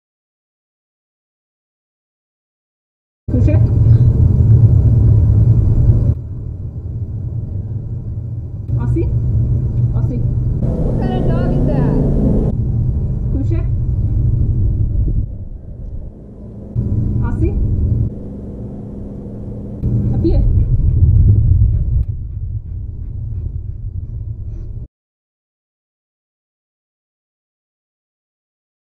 Preview Basic Malinois Puppy Commands in French - Obedience Dog Training Piper Houston Texas 2017
We go over basic dog training commands in French